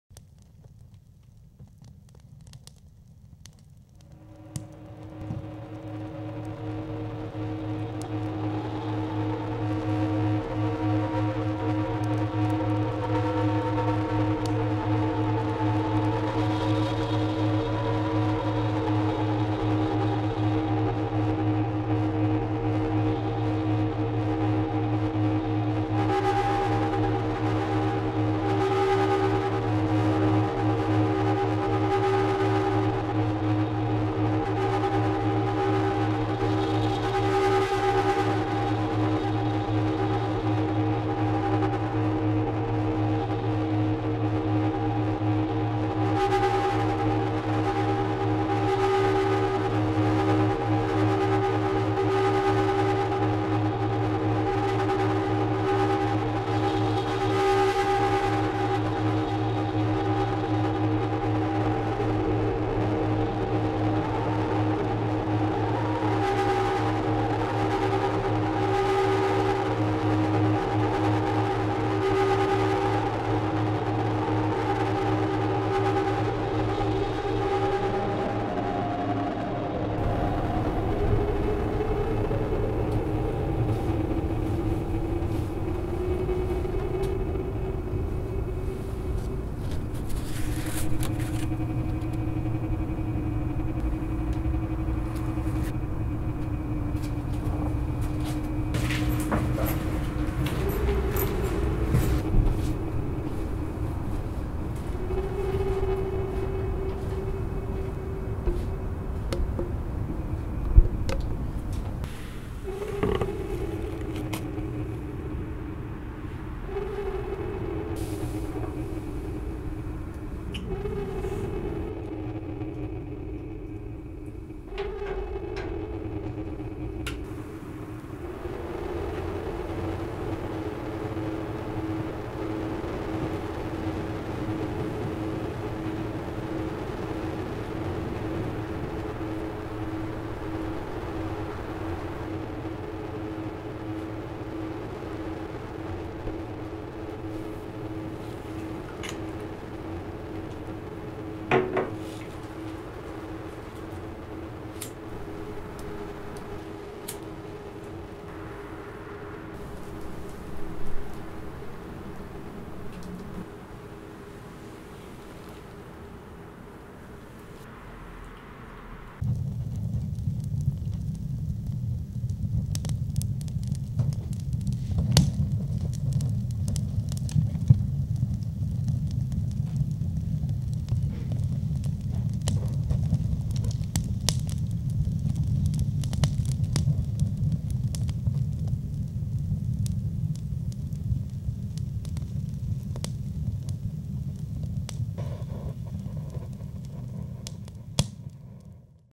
Wishes
Court métrage réalisé par Thai-Binh Phan-Van Masanobu et Maxime Ducharme dans le cadre d'un workshop organisé par Louidgi Beltrame et Christophe Acker.
Avec Gentaro Murakami, Emilie Lange, Thai-Binh Phan-Van et Maxime Ducharme